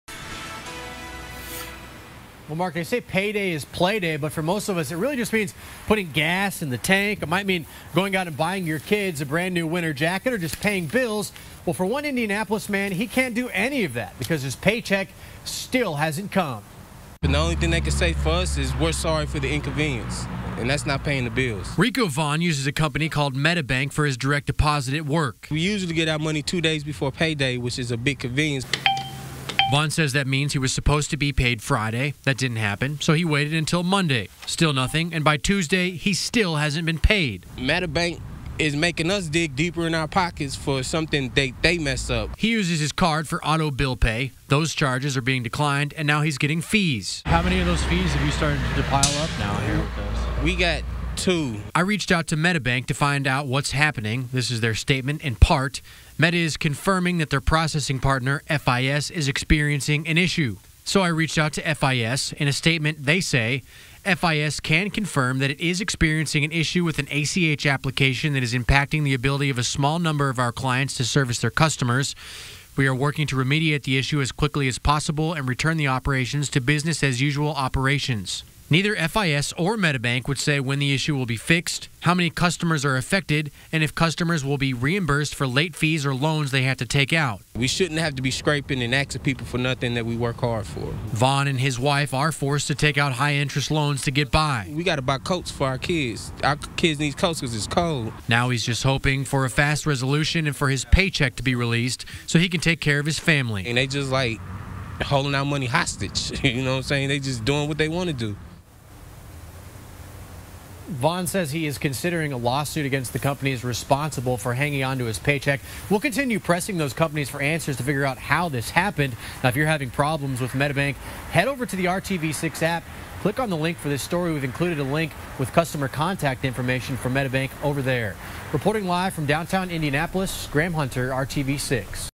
Man says company 'holding our money hostage'
Rico Vaughn said his paycheck is days late. The bank said its processing partner is dealing with an issue.